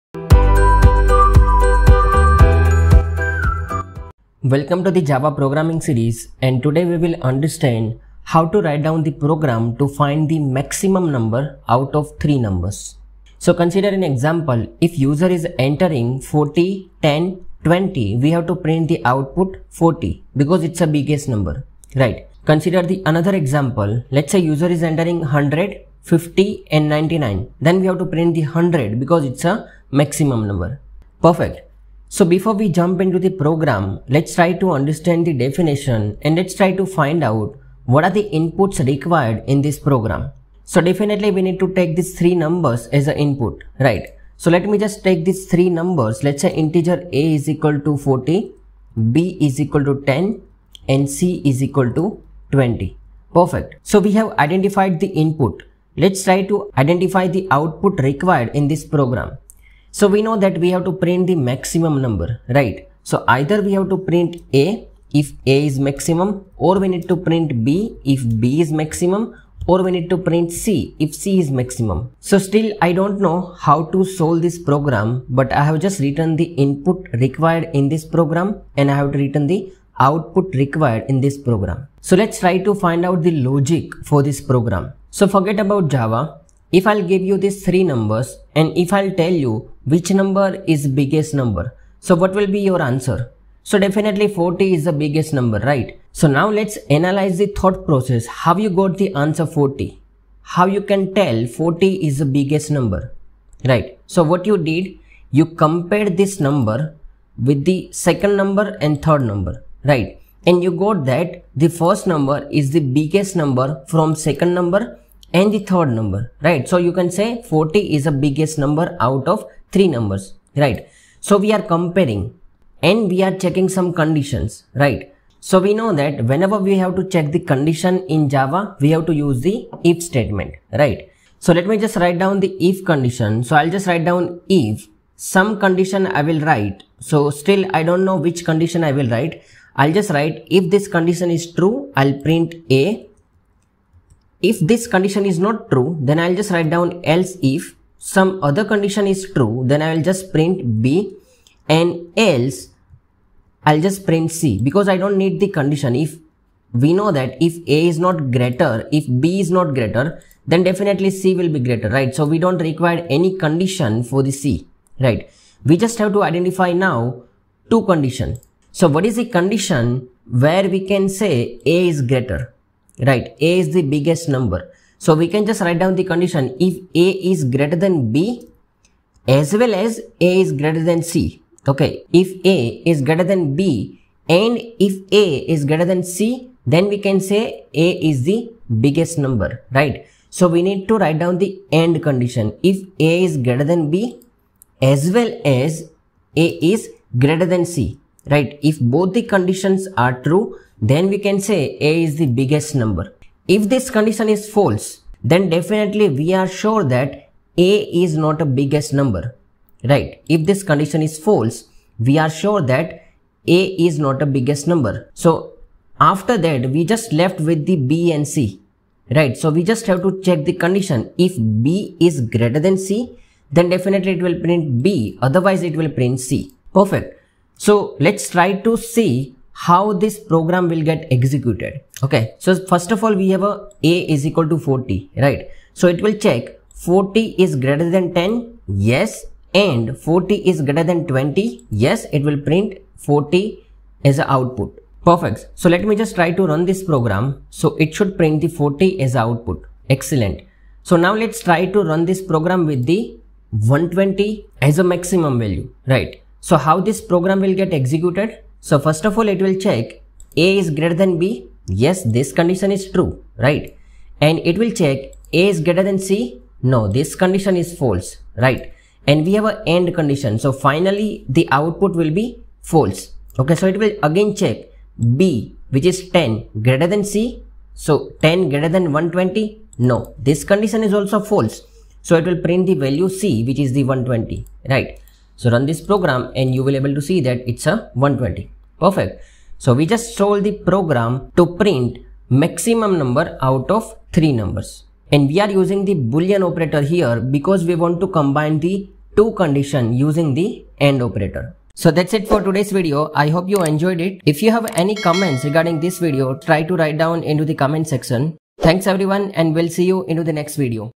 10 | Java Program To Find Maximum Number Out of Three Numbers | Java Boolean Operator
Preparing for the java interview or exam? Checkout my color-coded Java Revision Book that is specially designed for refreshing java concepts